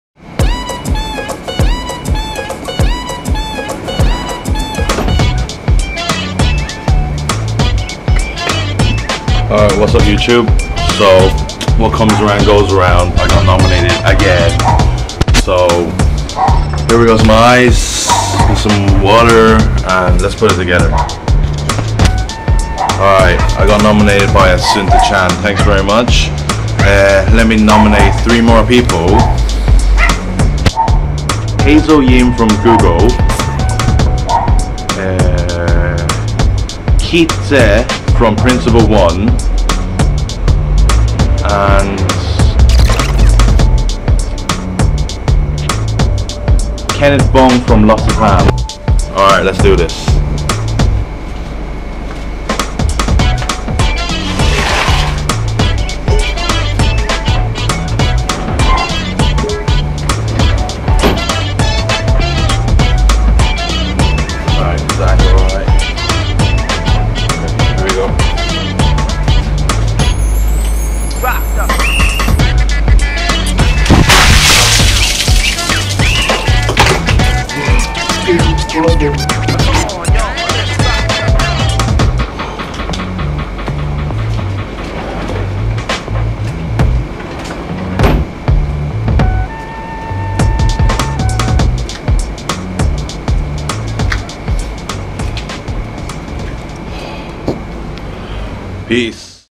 Ice Bucket Challenge 2
Got nomiated again, so to keep this going I done it again and nominated 3 more people. You 3 have 24 hours do the challenge pass it onto another 3 and donate please.